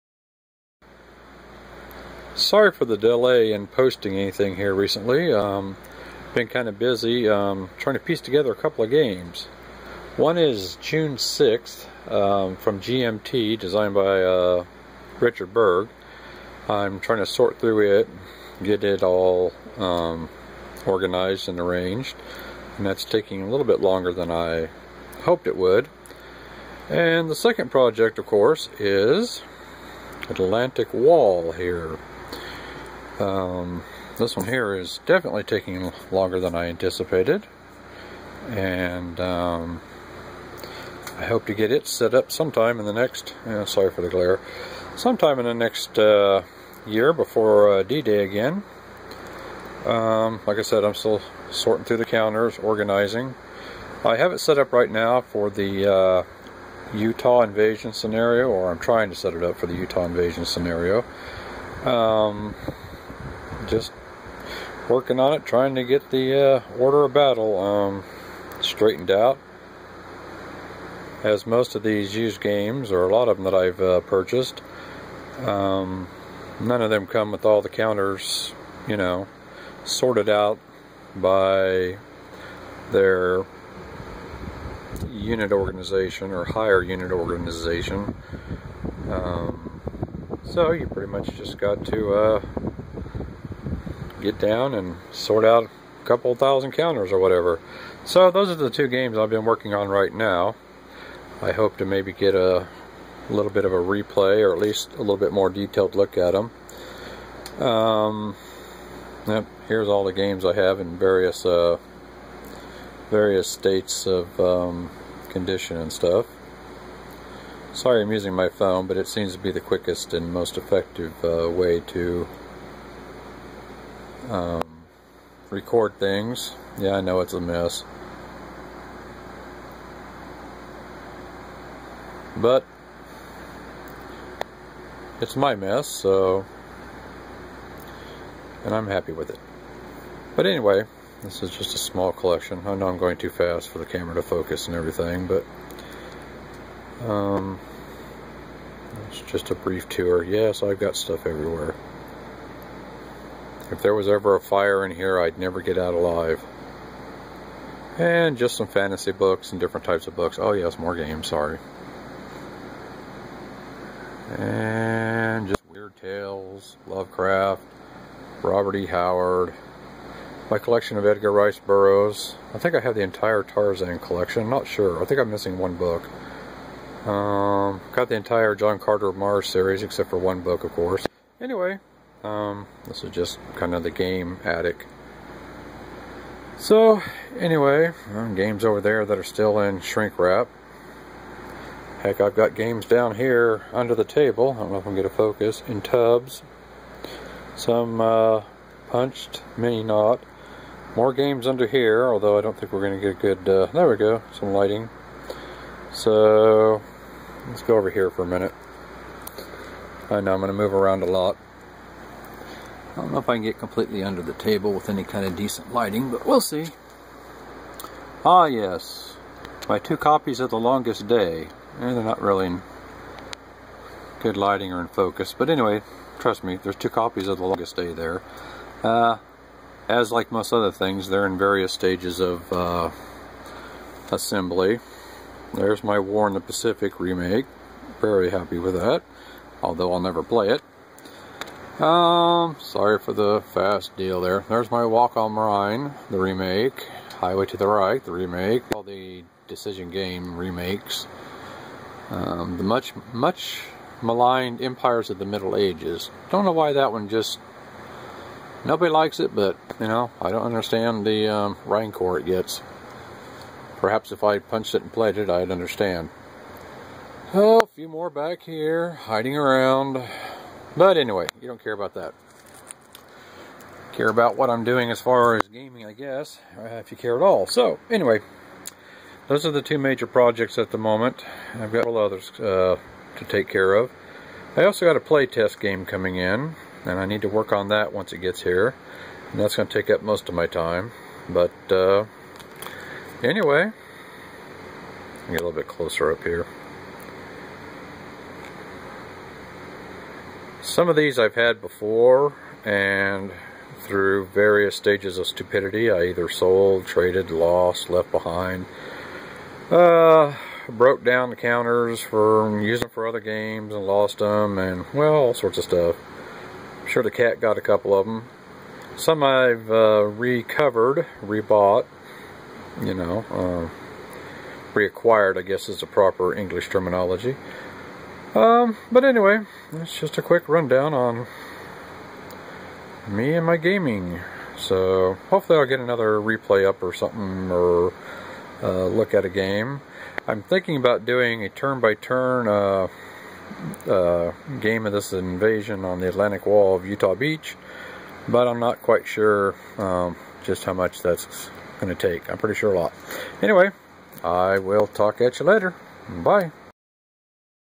Gaming Update 1/2
Just a brief look at games that I'm working on and a tour of my game room.